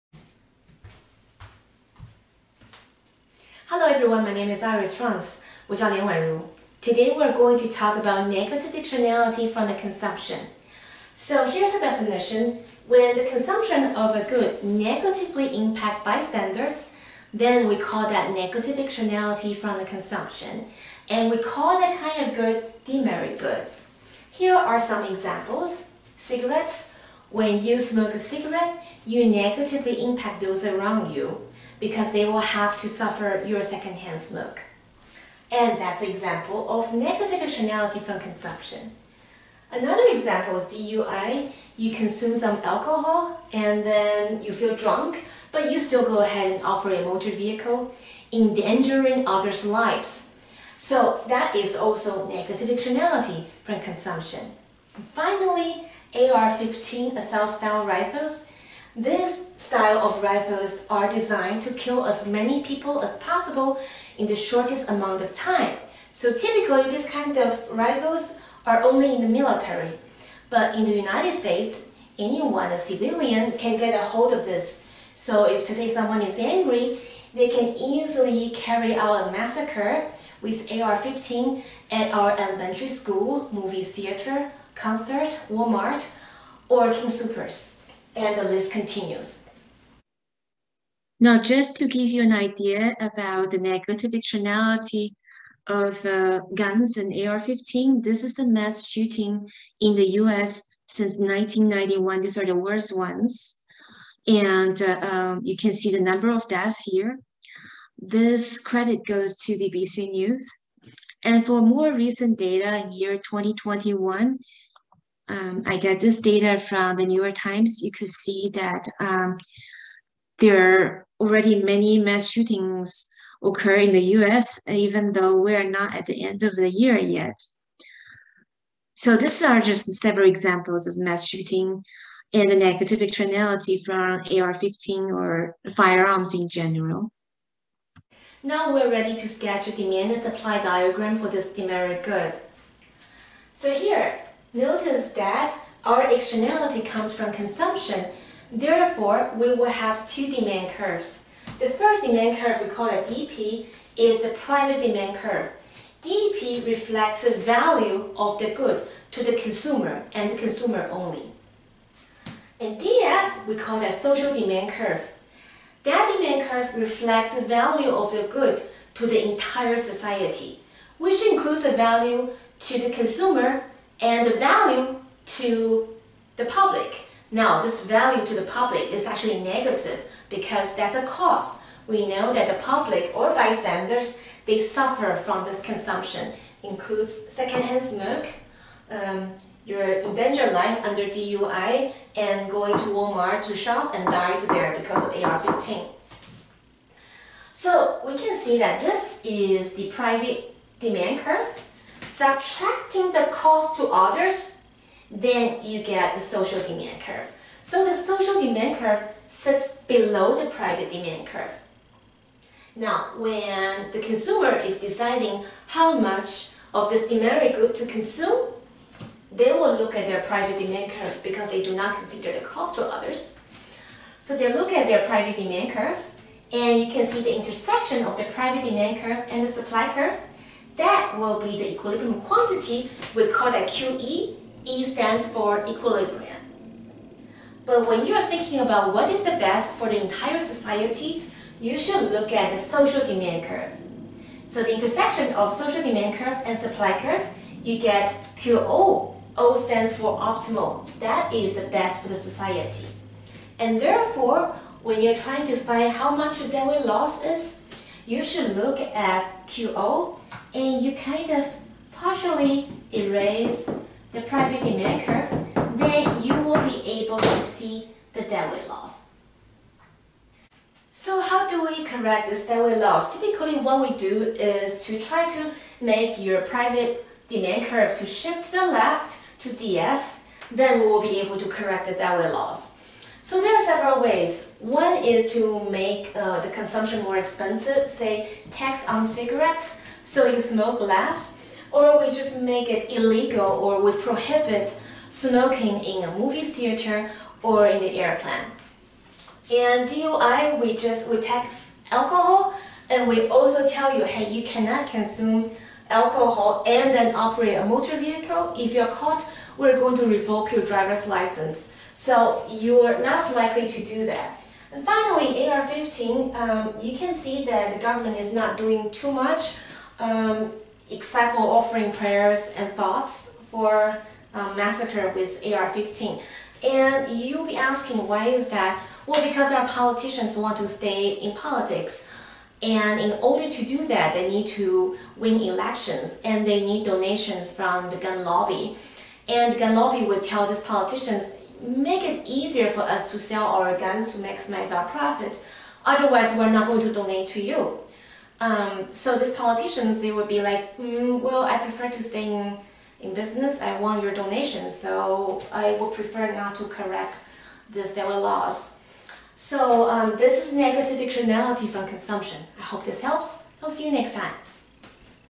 Negative Externality from Consumption (De-Merit Goods)
Negative externality of consumption occurs when the consumption of a good negatively impact others. Examples include cigarettes, DUI, and AR-15 assault style rifles.